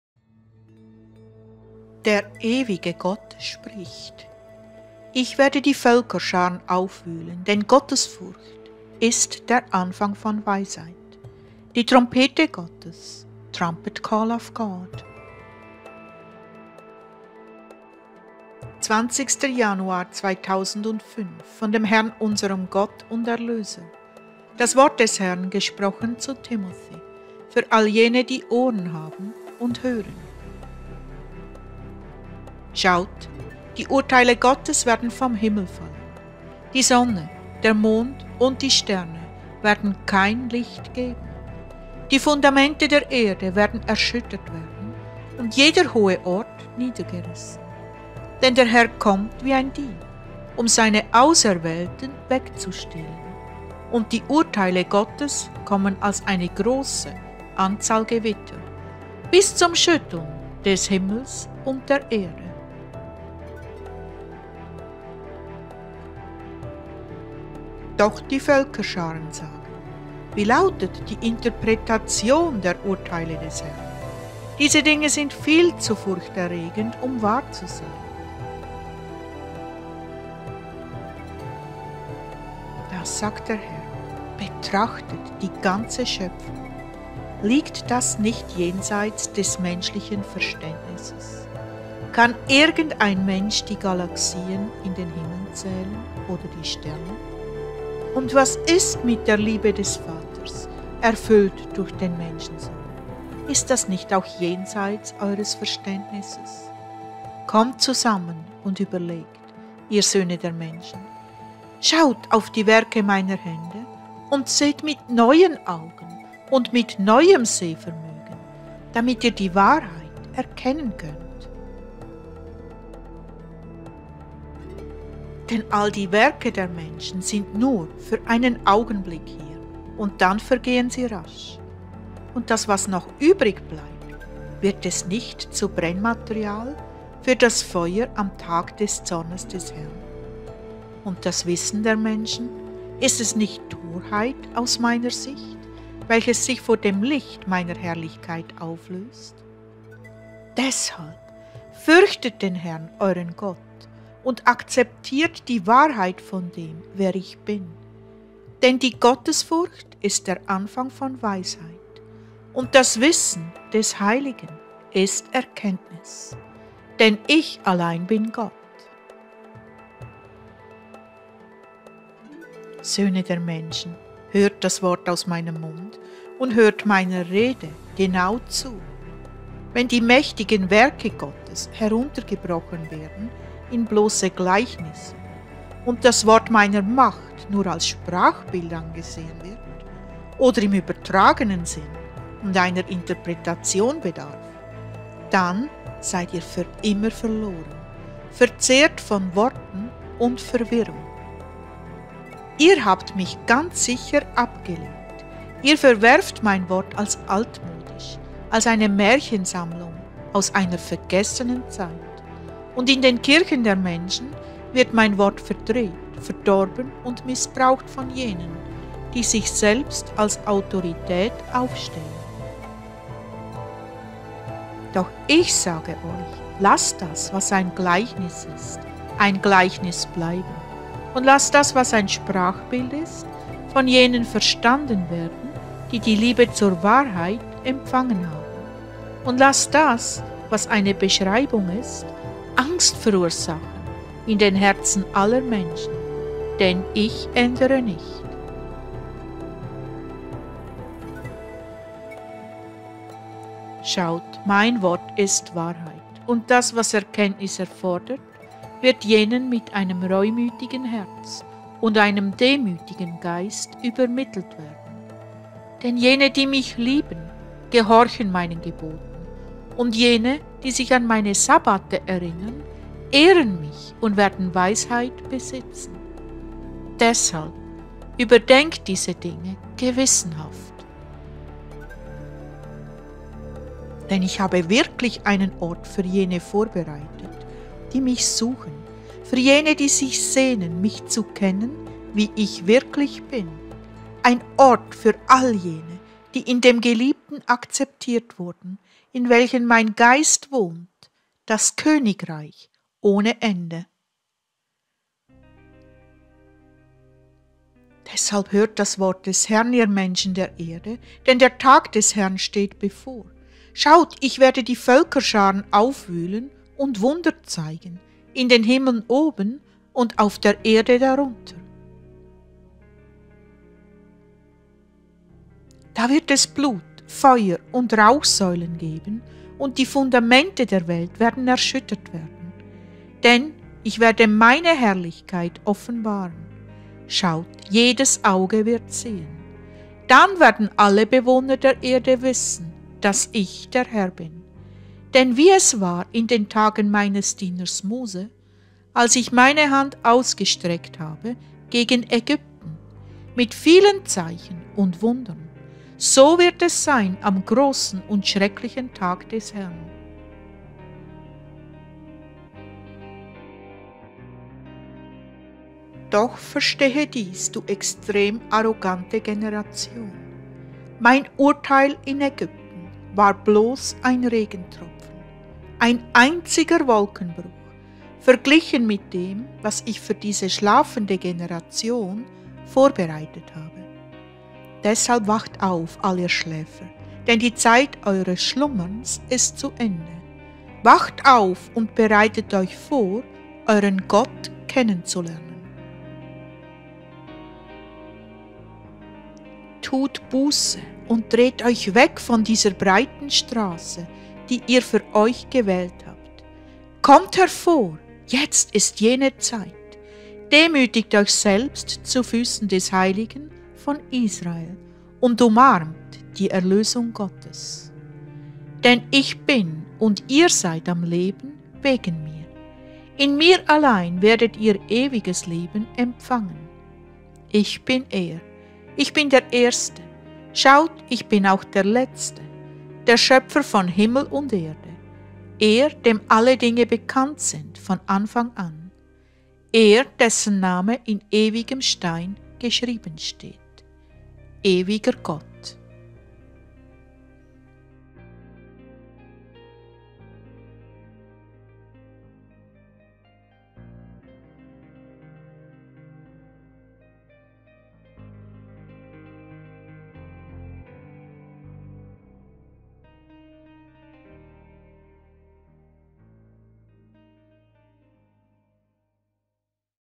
Rhema 21.05.2022 🎺 Gottesfurcht ist der Anfang von Weisheit... Ich werde die Völkerscharen aufwühlen
Der Ewige Gott spricht… Gottesfurcht ist der Anfang von Weisheit!…
Ich werde jetzt die Völkerscharen aufwühlen

20. Januar 2005 – Von Dem Herrn, Unserem Gott und Erlöser – Das Wort Des Herrn gesprochen zu Timothy, für all Jene, die Ohren haben und hören

Schaut die Urteile Gottes werden vom Himmel fallen! Die Sonne, der Mond und die Sterne werden kein Licht geben! Die Fundamente der Erde werden erschüttert werden und jeder hohe Ort niedergerissen! Denn Der Herr kommt wie ein Dieb, um Seine Auserwählten wegzustehlen und die Urteile Gottes kommen als eine grosse Anzahl Gewitter, bis zum Schütteln des Himmels und der Erde!

Doch die Völkerscharen sagen… ‚Wie lautet die Interpretation der Urteile des Herrn? Diese Dinge sind viel zu furchterregend, um wahr zu sein!‘

Das sagt Der Herr… Betrachtet die ganze Schöpfung! Liegt das nicht jenseits des menschlichen Verständnisses? Kann irgend ein Mensch die Galaxien in den Himmeln zählen oder die Sterne? Und was ist mit der Liebe des Vaters, erfüllt durch den Menschensohn? Ist das nicht auch jenseits eures Verständnisses? Kommt zusammen und überlegt, ihr Söhne der Menschen. Schaut auf die Werke Meiner Hände und seht mit neuen Augen und mit neuem Sehvermögen, damit ihr die Wahrheit erkennen könnt.

Denn all die Werke der Menschen sind nur für einen Augenblick hier und dann vergehen sie rasch. Und das, was noch übrig bleibt, wird es nicht zu Brennmaterial für das Feuer am Tag Des Zornes Des Herrn? Und das Wissen der Menschen, ist es nicht Torheit aus Meiner Sicht, welches sich vor dem Licht Meiner Herrlichkeit auflöst?… Deshalb, fürchtet den Herrn euren Gott und akzeptiert die Wahrheit von dem, wer Ich bin! Denn die Gottesfurcht ist der Anfang von Weisheit und das Wissen des Heiligen ist Erkenntnis. Denn Ich allein bin Gott.

Söhne der Menschen, hört das Wort aus Meinem Mund und hört Meiner Rede genau zu… Wenn die mächtigen Werke Gottes heruntergebrochen werden in blosse Gleichnisse und das Wort Meiner Macht nur als Sprachbild angesehen wird oder im übertragenen Sinn und einer Interpretation bedarf, dann seid ihr für immer verloren, verzehrt von Worten und Verwirrung… Ihr habt Mich ganz sicher abgelehnt! Ihr verwerft Mein Wort als altmodisch, als eine Märchen-Sammlung aus einer vergessenen Zeit! Und in den Kirchen der Menschen wird Mein Wort verdreht, verdorben und missbraucht von Jenen, die sich selbst als Autorität aufstellen!

Doch Ich sage euch, lasst das, was ein Gleichnis ist, ein Gleichnis bleiben und lasst das, was ein Sprachbild ist von Jenen verstanden werden, die die Liebe zur Wahrheit empfangen haben und lasst das, was eine Beschreibung ist, Angst verursachen in den Herzen aller Menschen! Denn Ich ändere nicht!

Schaut, Mein Wort ist Wahrheit!… Und das, was Erkenntnis erfordert, wird Jenen mit einem reumütigen Herz und einem demütigen Geist übermittelt werden. Denn Jene, die Mich lieben, gehorchen Meinen Geboten und Jene, die sich an Meine Sabbate erinnern, ehren Mich und werden Weisheit besitzen… Deshalb überdenkt diese Dinge gewissenhaft…

Denn Ich habe wirklich einen Ort für Jene vorbereitet, die Mich suchen, für Jene die sich sehnen, Mich zu kennen, wie Ich wirklich bin, ein Ort für all Jene, die in Dem Geliebten akzeptiert wurden, in welchen Mein Geist wohnt, das Königreich ohne Ende…

Deshalb hört Das Wort Des Herrn, ihr Menschen der Erde, denn Der Tag Des Herrn steht bevor… Schaut, Ich werde die Völkerscharen aufwühlen und Wunder zeigen in den Himmeln oben und auf der Erde darunter!

LINK ZUM TEXT GANZ OBEN